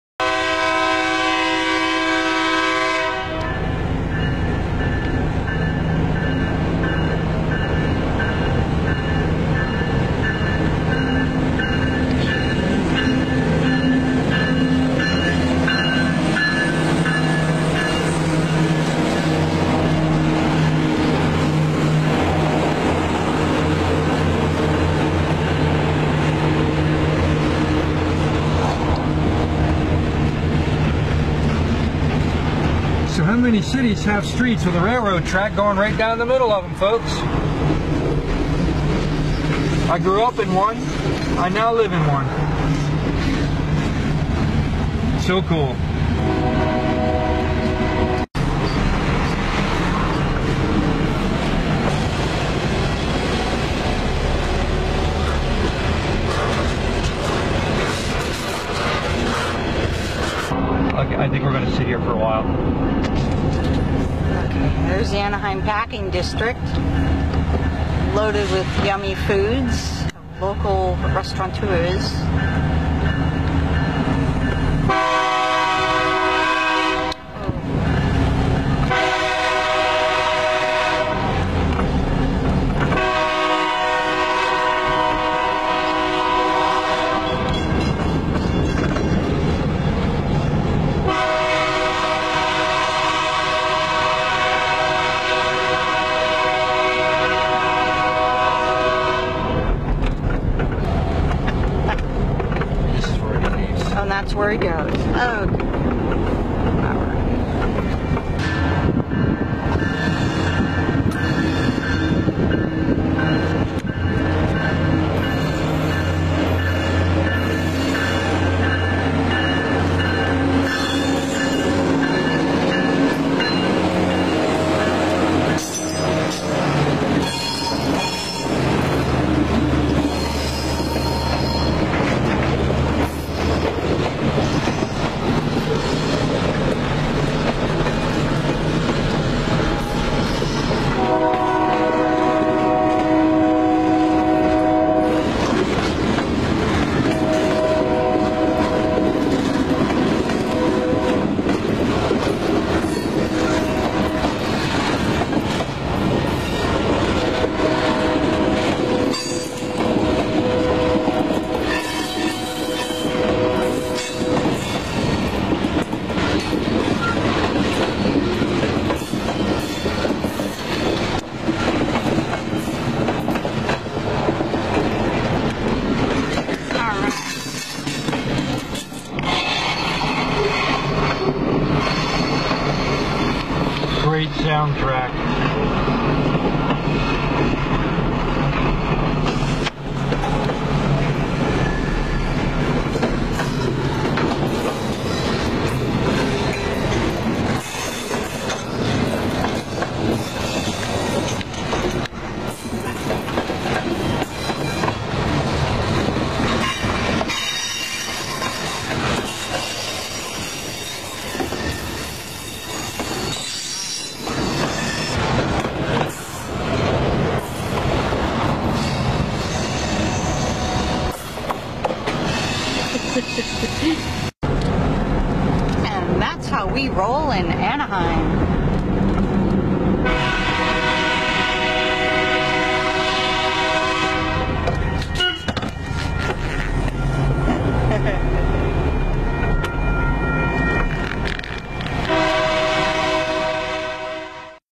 Tan 1967 VW "Races" Santa Fe Train 2742 | VW Bruce
It was a hot summer day in 2014 when we happened upon a Santa Fe Train 2742 literally cruising through the legendary Anaheim Packing District! We kept pace in our desert beige 1967 Volkswagen for a fun "E-Ticket" ride! TURN UP the volume. The train blasts are, well, a blast! Let's ride!! God Bless!